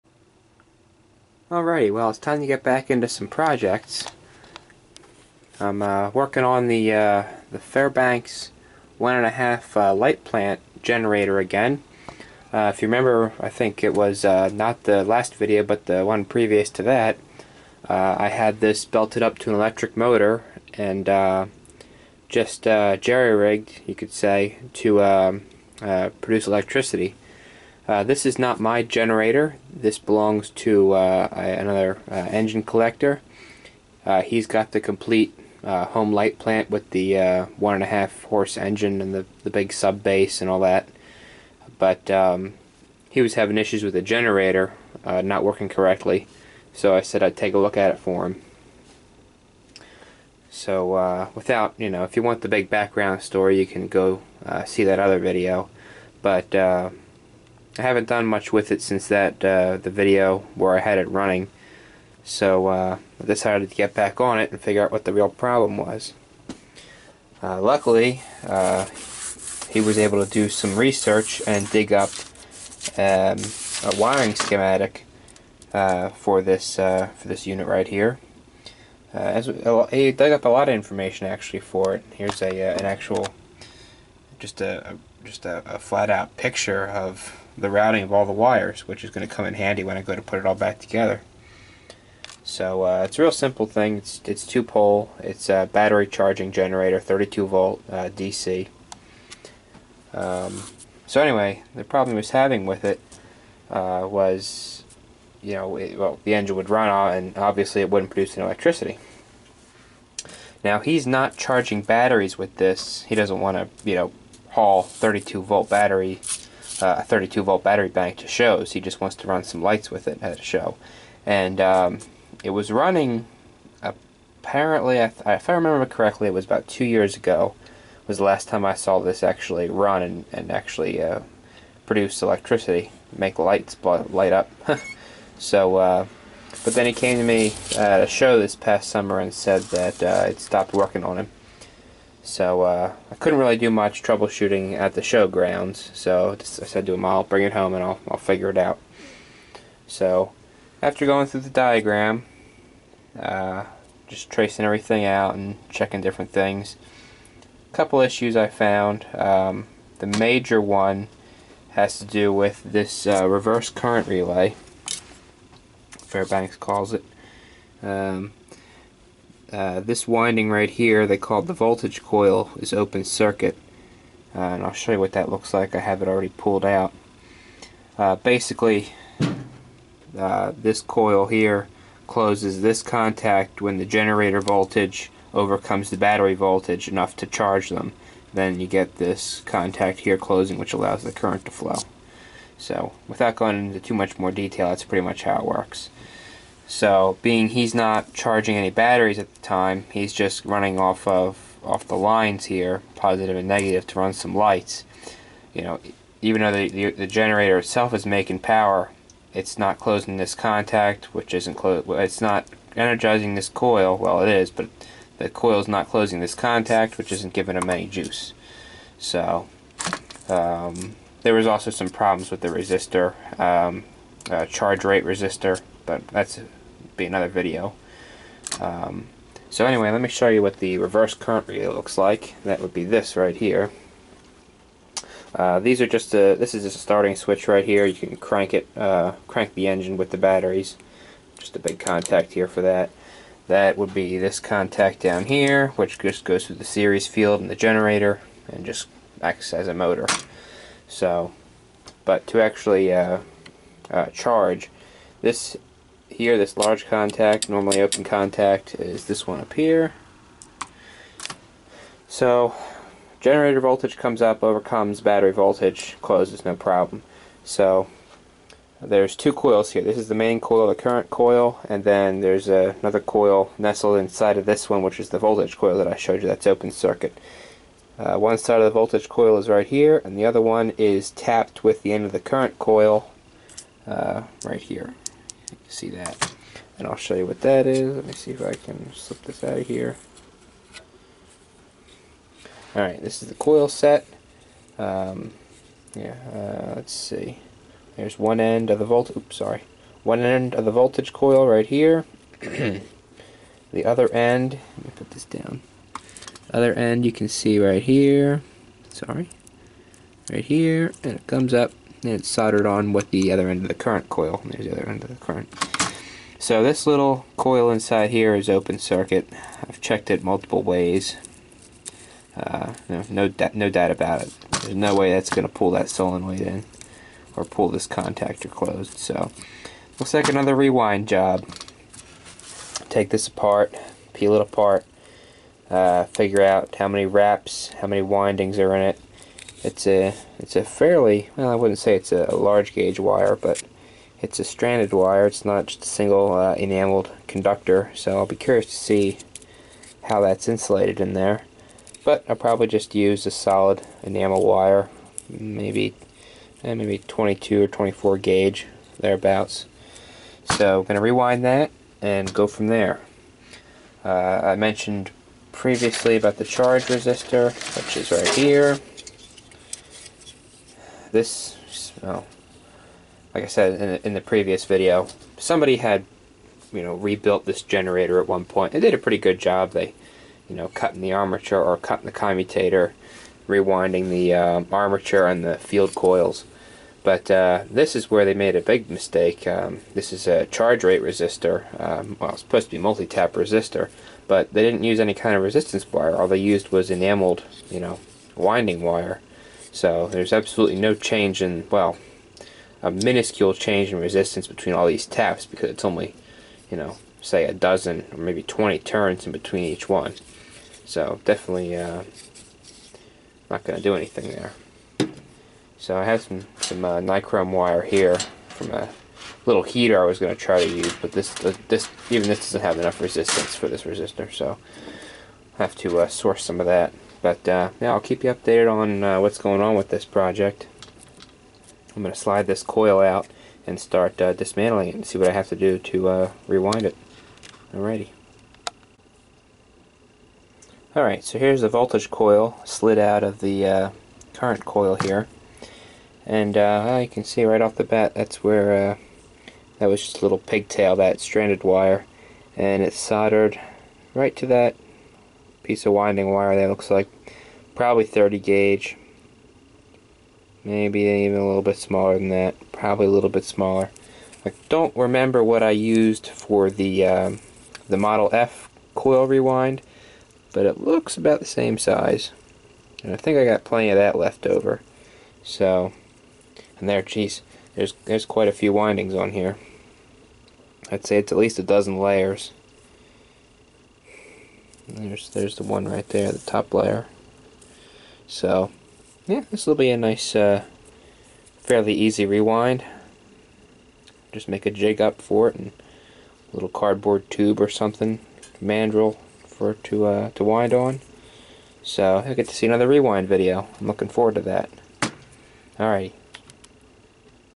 (PART 2) Fairbanks Morse Light Plant Generator
Time to do some more work on the Fairbanks  1 1/2  home light pant generator.